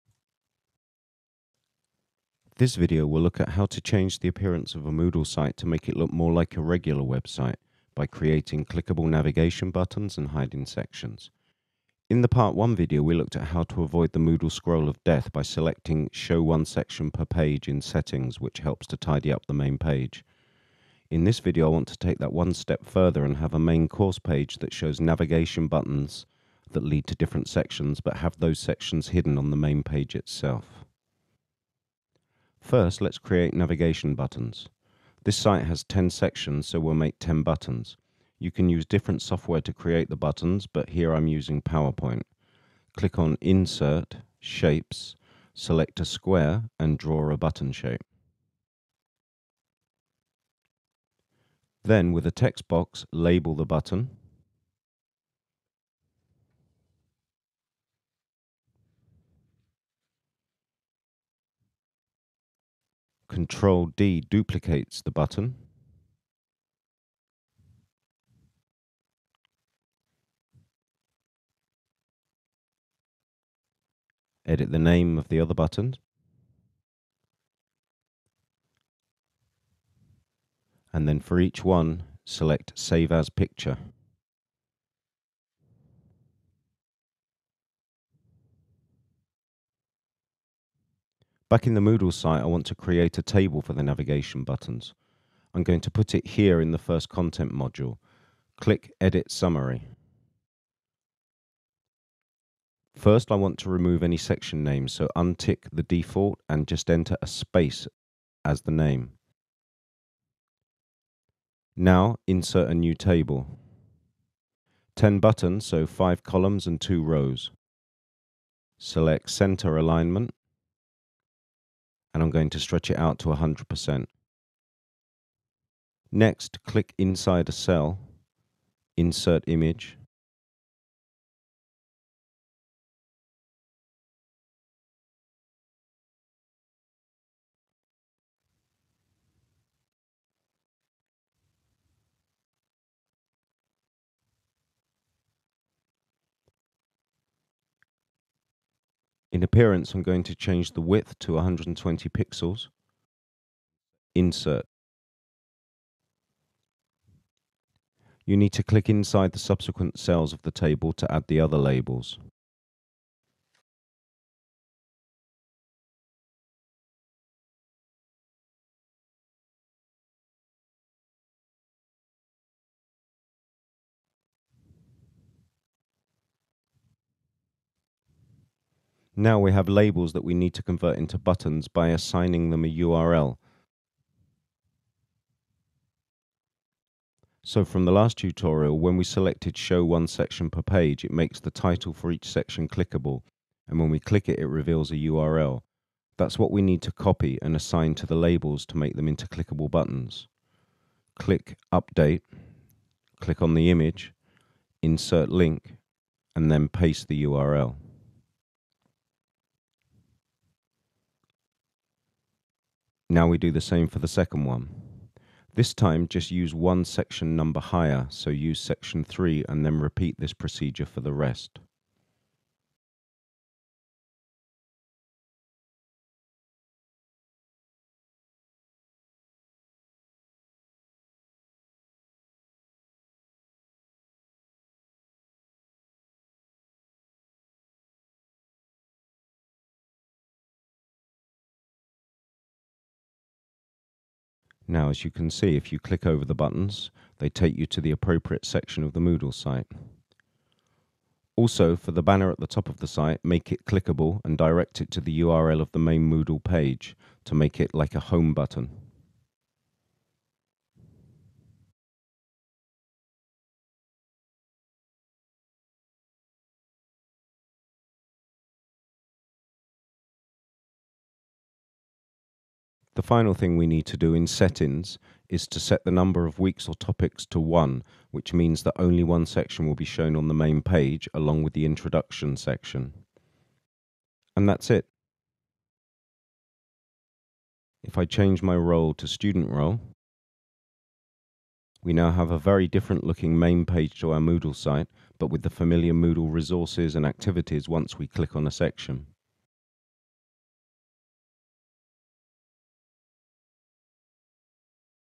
Customising Moodle Part 2: Giving it a website-like appearance
Customising a Moodle 2.3 site by adding navigation buttons and hiding sections